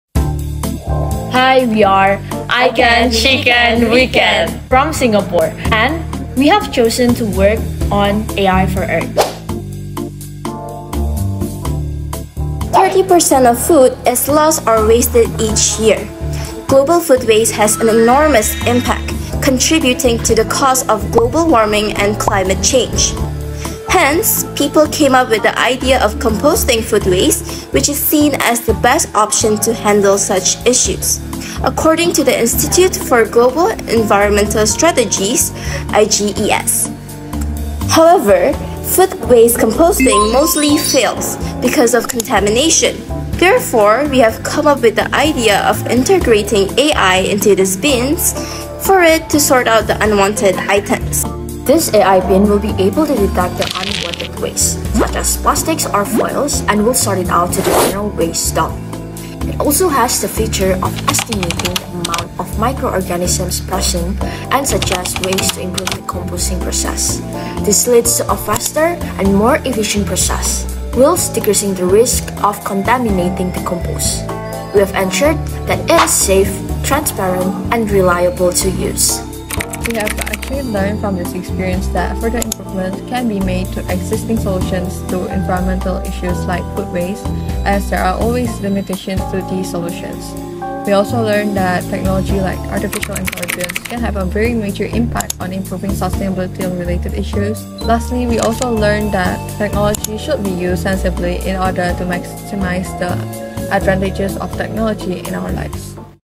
Imagine Cup Junior 2023
Submitting school:  Methodist Girls’ School
Student team name:  I Can, She Can, We Can
The idea in a sentence: Detecting unwanted items in food waste bins, ensure success of food composting.
Number of team members:  3
AI for Good initiative:  AI for Earth